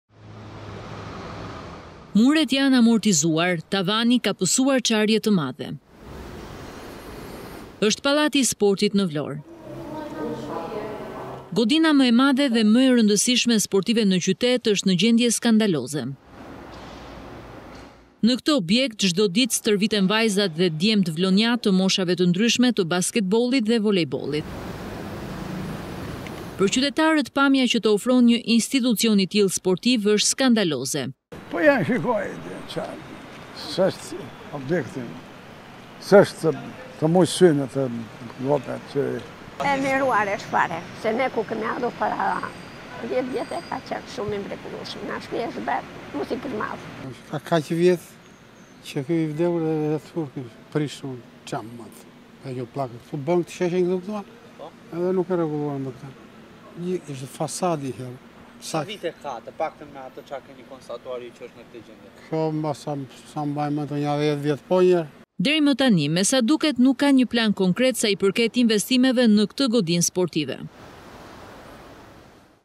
Pallati i sportit në Vlorë i amortizuar e pa asnjë kusht
Pallati i Sportit në Vlorë ndodhet në një gjendje jashtëzakonisht të keqe, për shkak të mungesë së investimeve. Muret e Pallatit janë amortizuar, ndërsa tavani ka nisur duke pësuar çarje të mëdha, ndërsa edhe në këto kushte ende nuk ka asnjë sinjal për investime serioze atje.

Muret janë amortizuar, tavani ka pësuar çarje të mëdha.

Ky është pallati i Sportit në Vlorë.

Godina më e madhe dhe më e rëndësishme sportive në qytet, është në gjendje skandaloze.

Në këtë objekt çdo ditë stërviten vajzat dhe djemtë vlonjatë të moshave të ndryshme të basketbollit dhe volejbollit.

KETU LAJMET MINUTE PAS MINUTE

Për qytetarët pamja që të ofron një institucion i tillë sportive është skandaloze.

"Po shikoje vet. S'është për tu parë e me të kënaq syrin.

Para 10 vjetësh ishte i mrekullueshëm, tani është bërë si gërmadhë.

Ka kaq vite që është i prishur, me xhama të thyer. Rregulluan këtë sheshin këtu dhe nuk e bënë më këtë," thonë qytetarët.

Deri më tani mesa duket nuk ka një plan konkret sa i përket inmvestimeve në këtë godinë sportive.